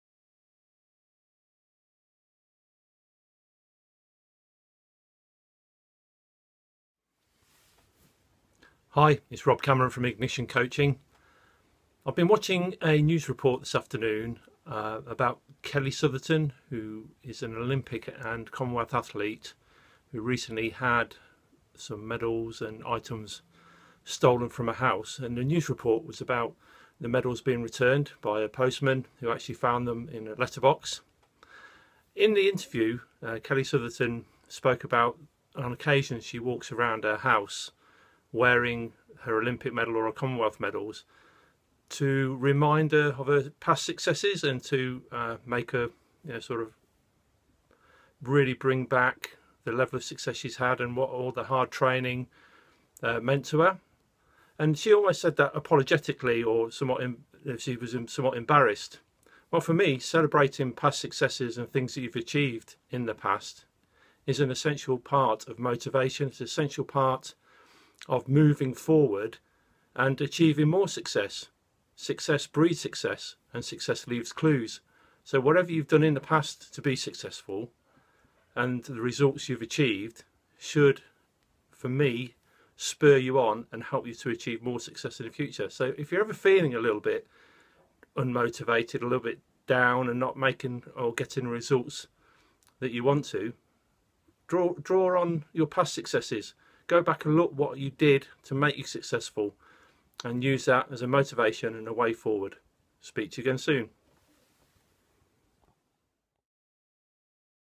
[Ignition Insights] Success leaves clues
In a TV program I was watching Kelly Sotherton, a retired Commonwealth and Olympic athlete, was featured. Kelly had her medals stolen and was featured having them returned by the Postman that found them. In the interview Kelly spoke about times when she had worn her medals around the house to motivate her and remind her about the fruits of her hard work & training.
I believe that calling on past achievements and successes is essential to future success; it can provide you with motivation and highlight personal attributes & skills that can propel you forward